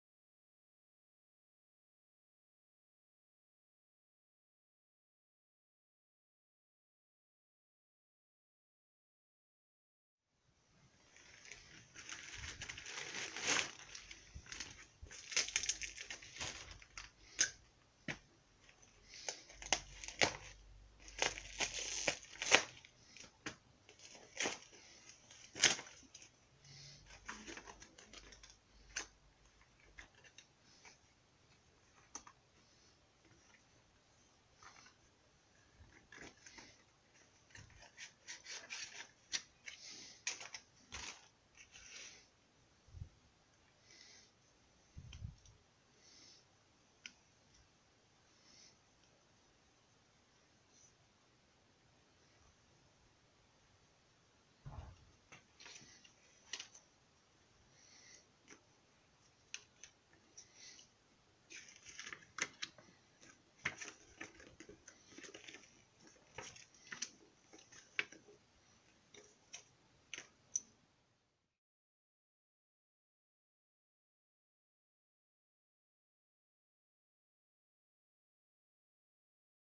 Comfast 923AC from BangGood
Comfast 923AC Dual Band 2.4G 5.8G 600Mbps Bidirectional USB Wifi Dongle Wireless Networking Adapter.
(It is really big for put it especially in laptop but has very very
strong singal.)